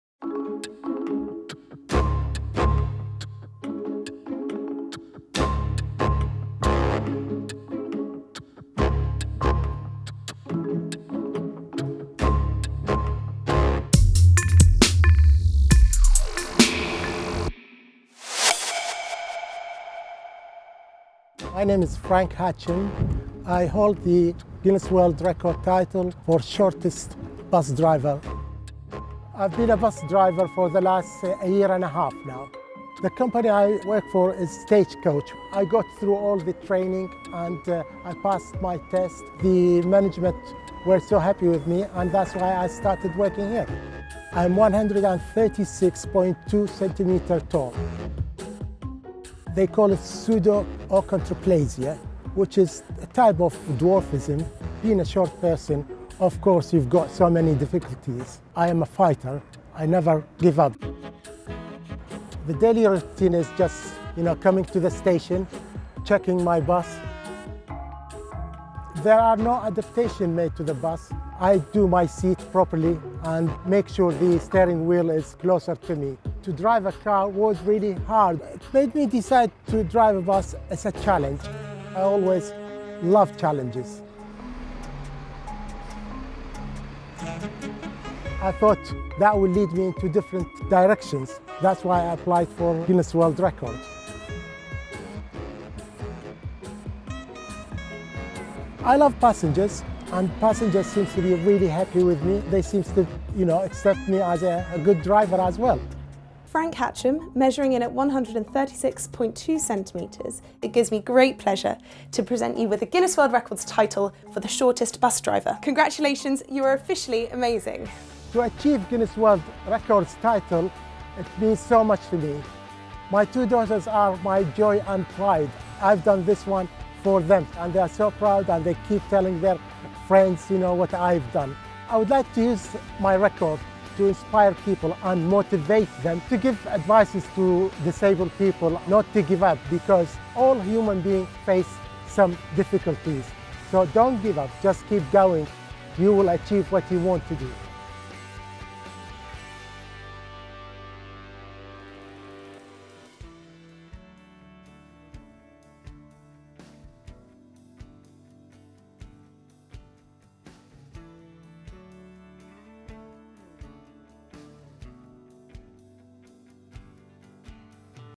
Frank Hachem: World's Shortest Bus Driver - Guinness World Records
Frank Faeek Hachem works in Chichester, West Sussex, UK, and now holds the Guinness World Records title for the Shortest bus driver. Frank has achondroplasia, a common form of dwarfism, but he has never let his height hold him back.